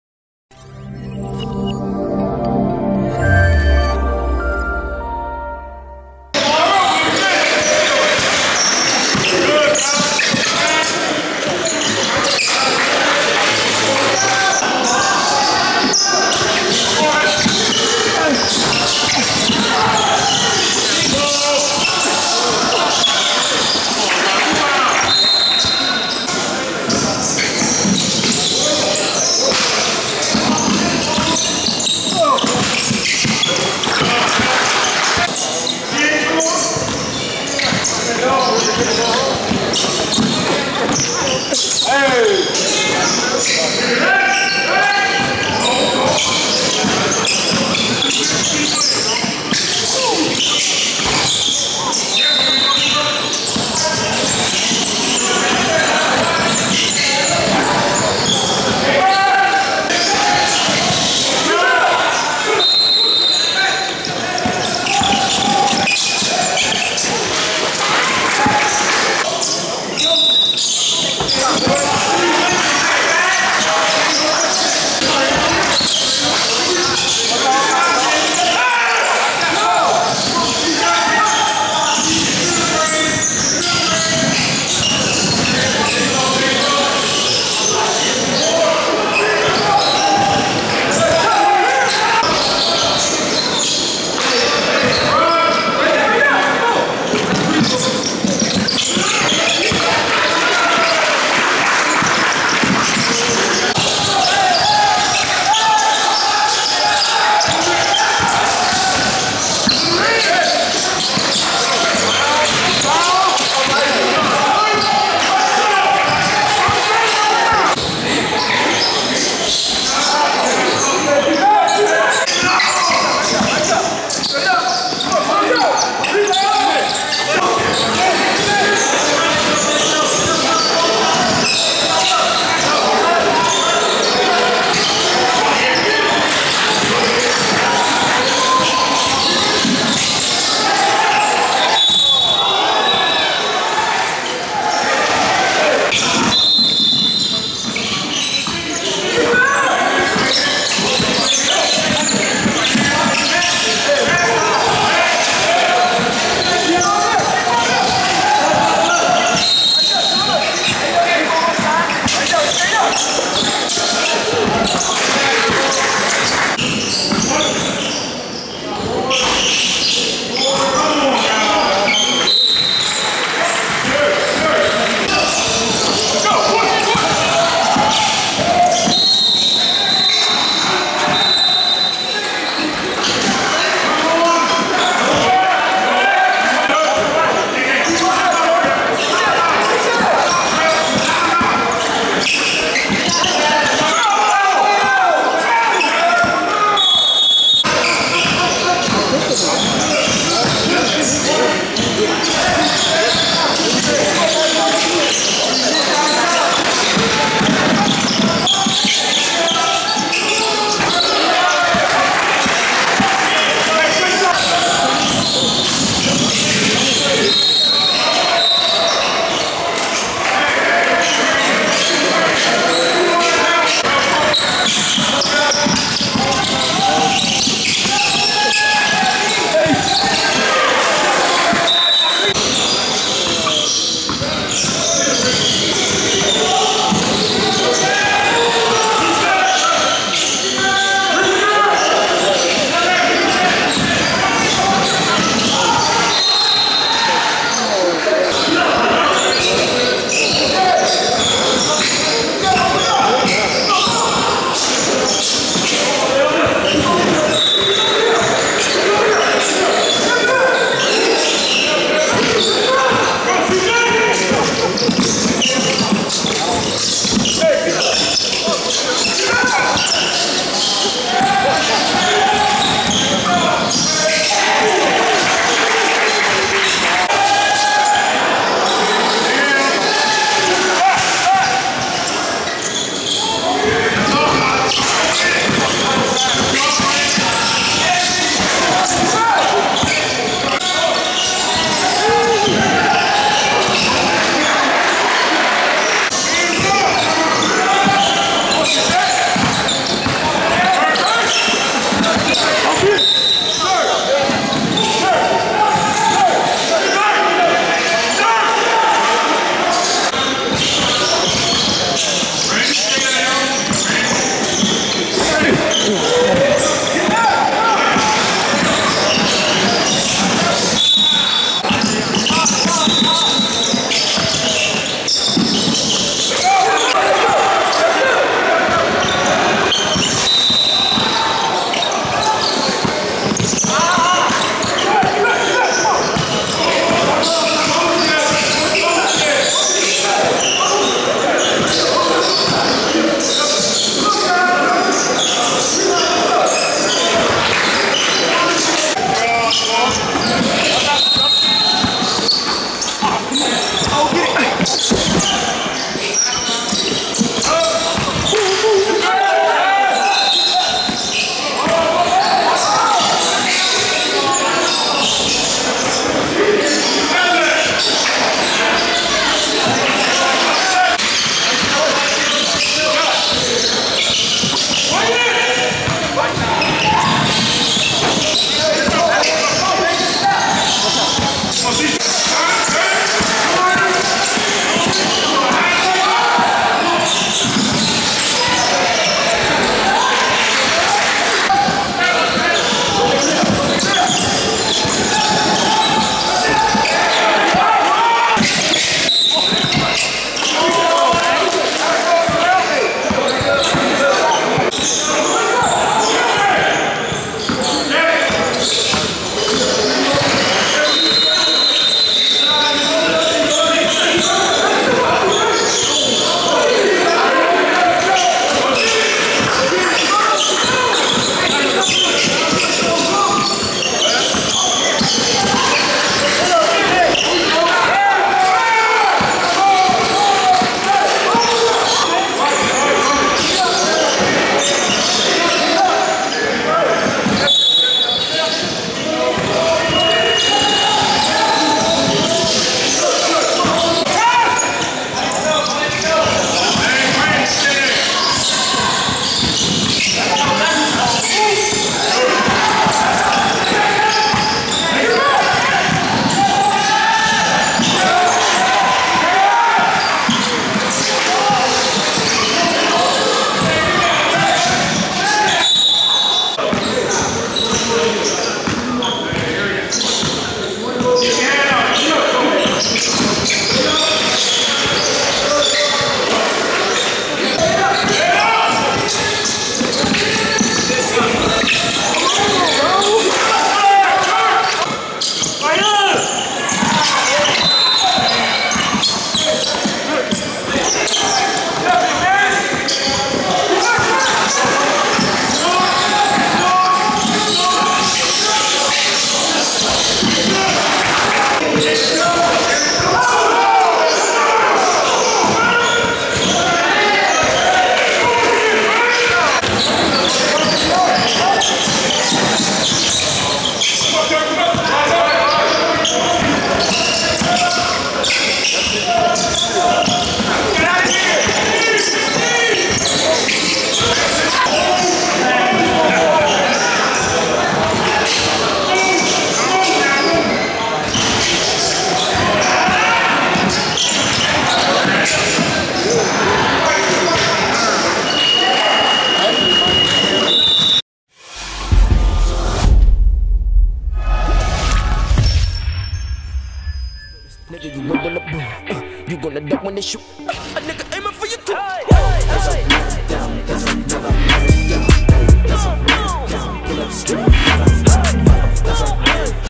Sir Issac Herron vs Carlous "CJ" Williams !!!UAA vs 3SSB!!!
17U battle of shoe circuit programs of Under Armor against Adidas. Houston Defenders (UAA) vs New Orleans Elite (3SSB) A lot of Scholarships on the court in this battle at The Elite 36 in Houston, Texas.